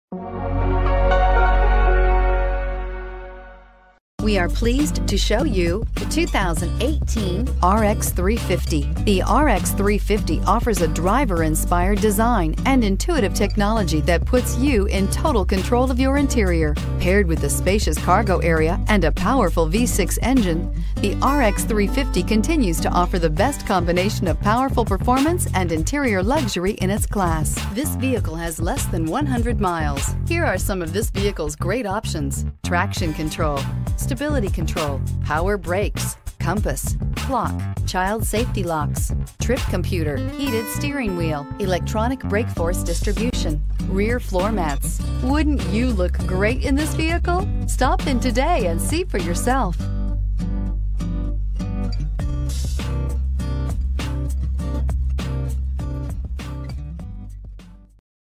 2018 Lexus RX 350 F Sport for sale in Durham NC
2018 Lexus RX 350 F Sport for sale in Durham NC - Stock # D35153
Year: 2018
Engine: 3.5L V6 DOHC 24V
Transmission: Automatic
Exterior: Ultra White
Interior: Rioja Red Leather With Scored Aluminum Trim
Drive Train: AWD
Cylinders: 6
Trim: F Sport
Address:
VIN: 2T2BZMCA9JC137001